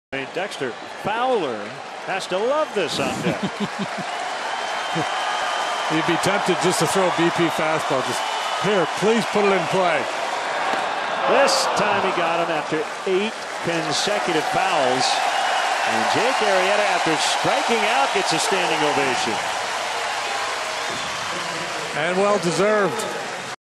PIT@CHC: Arrieta gets ovation after 14-pitch at-bat
Jake Arrieta receives a standing ovation from the Wrigley crowd after working a 14-pitch at-bat in the 4th inning